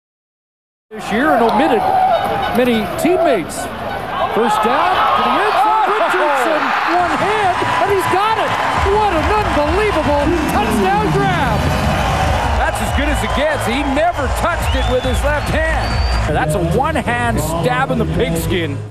CFL Richardson Stabs the Pigskin for the TD- Great Catch!
Montreal Receiver Richardson makes a huge 1 handed grab in the end-zone for the touchdown. against Winnipeg July 4, 2008